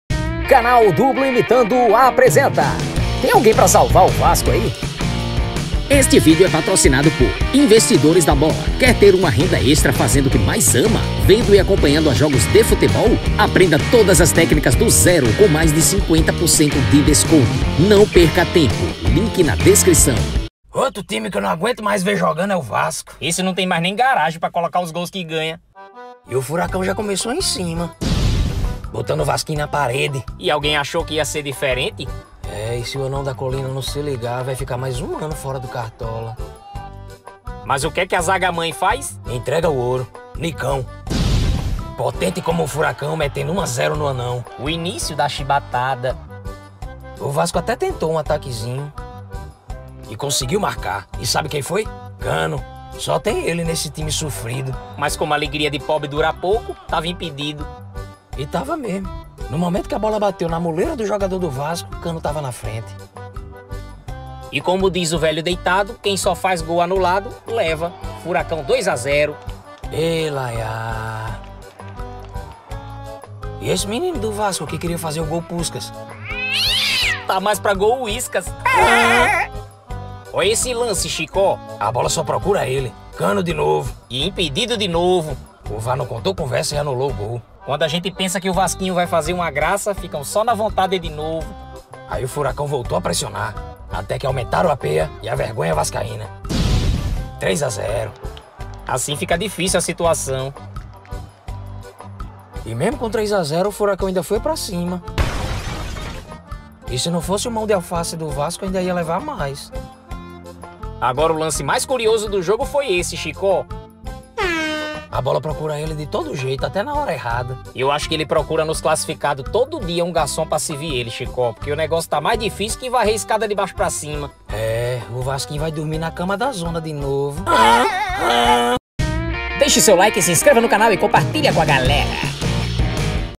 O Vasco leva outra pêa e continua na zona da degola | Athletico/PR 3 x 0 Vasco
Rapaz...não tem jeito mesmo. Alguém salve esse time kkk. O Vasco leva outra pêa e continua na zona da degola | Athletico/PR 3 x 0 Vasco. COMENTEM e COMPARTILHEM à vontade! Um abração, fiquem com Deus e valew!!!

Entre no canal do Telegram e tenha todos os vídeos pra compartilhar 😉.